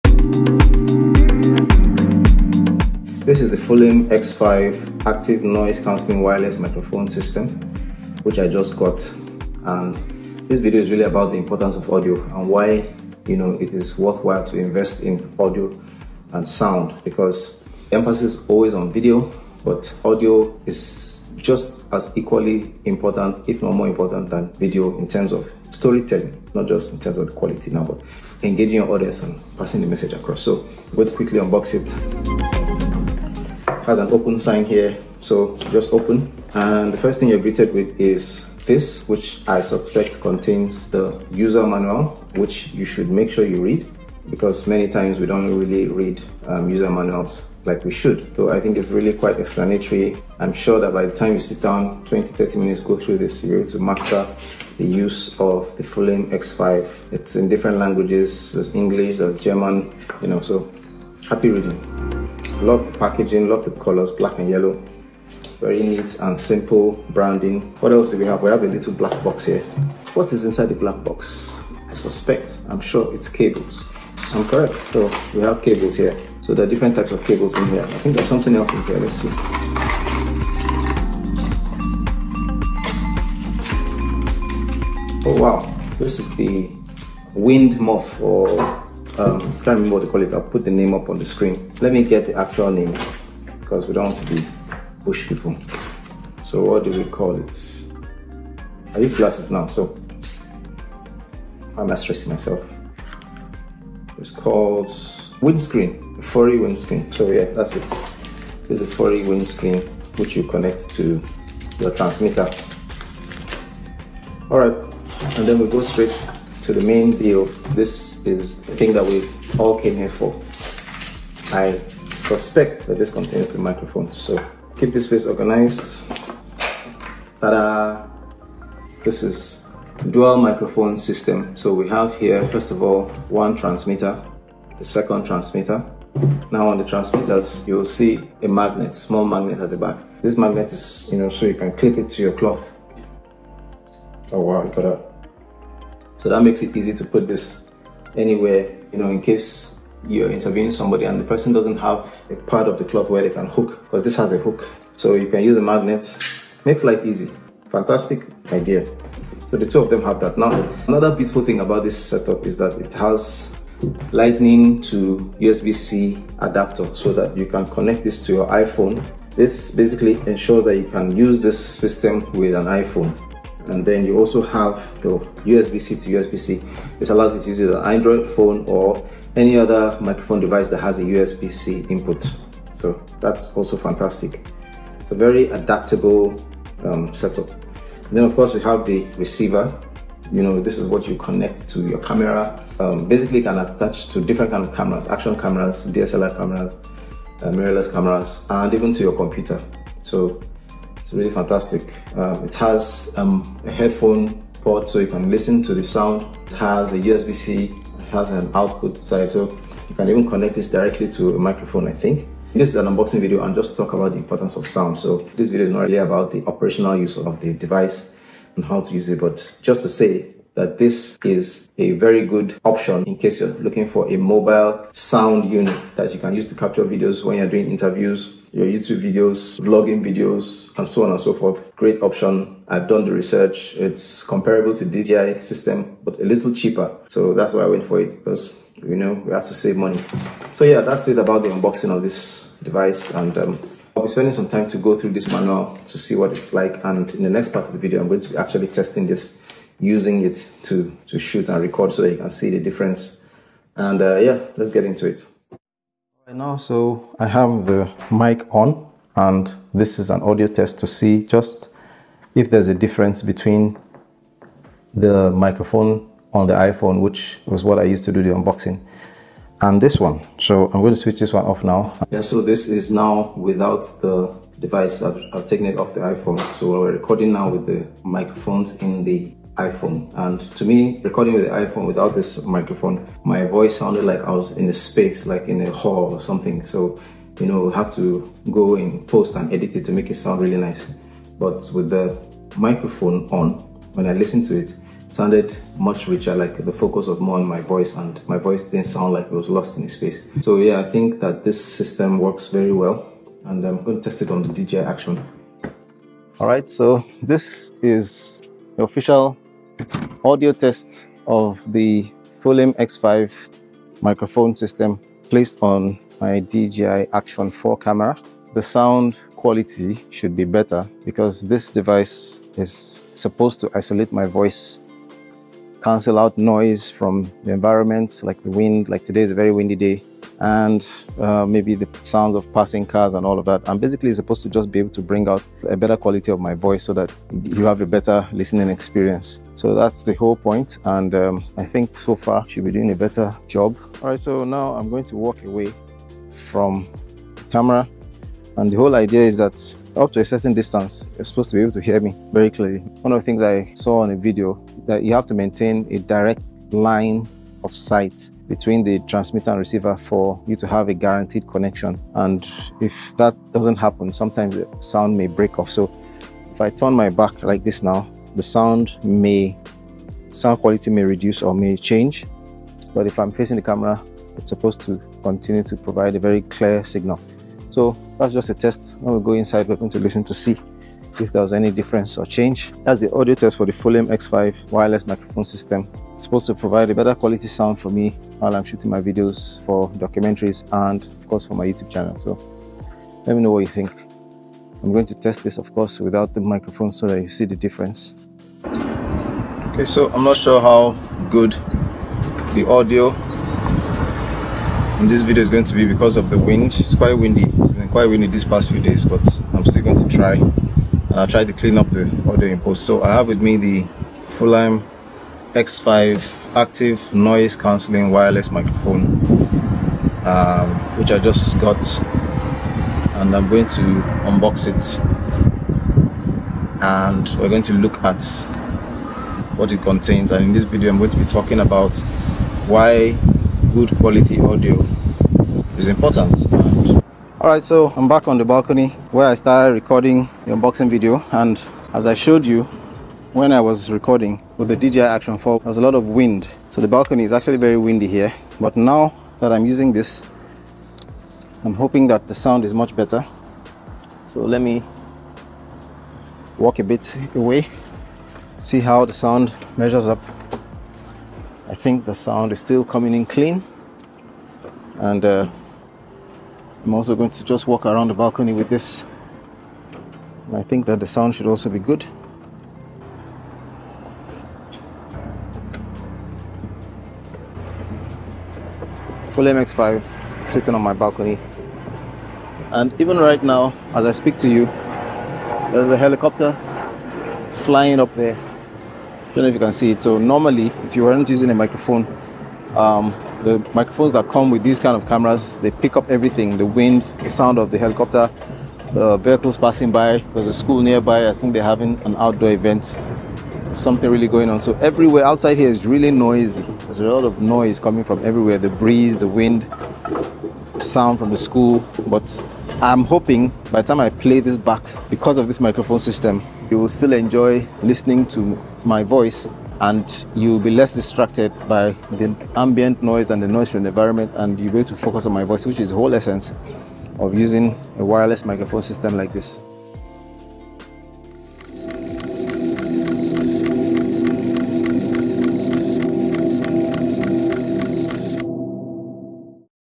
The Fulaim X5 Wireless Microphone: Unboxing and Testing
Looking for a game-changer in audio for filmmaking? Join me as I unboxing the Fulaim X5 Active Noise Cancelling Wireless Microphone System! We will be looking at the importance of audio and why investing in quality sound is essential for engaging your audience. With its sleek design and versatile features, the Fulaim X5 is a great choice for capturing professional-quality audio in any situation. As a video maker and content creator, I think adding this to your toolbox will greatly enhance your storytelling experience! It's ease-of-use, packaging, reliability and quality make it stand out and you definitely should consider it for documentaries, YouTube videos, vlogs and more.

It's very comparable in specs and performance to the dji wireless mics but more affordable!

If you are a video maker out there or YouTuber, do share what equipment you use tot capture audio in  your filmmaking process and what has been your best experience so far.